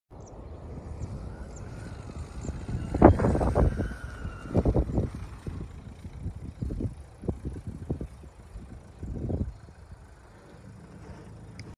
Launched by a car with a kite
The kite is a Vantage Vulture 14m² Gen3
Wind was like 10kts and car speed about 50-60km/h